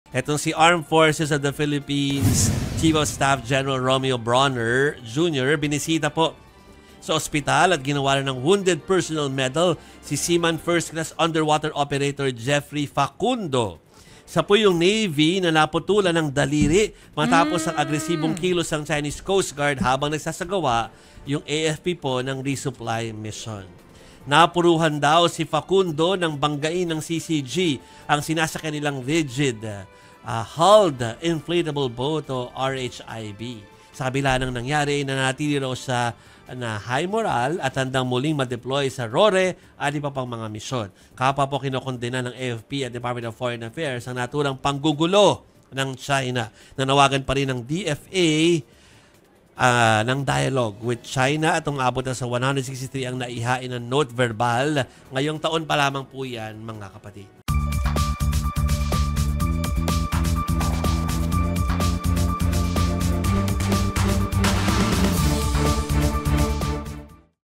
Sundalong naputulan ng daliri sa agresibong aksyon ng China, pinarangalan
WOUNDED PERSONNEL MEDAL

Ginawaran ng Wounded Personnel Medal si Seaman First Class Jeffrey Facundo na naputulan ng daliri sa agresibong aksyon ng China Coast Guard habang nagsasagawa ng resupply mission ang AFP sa Ayungin shoal.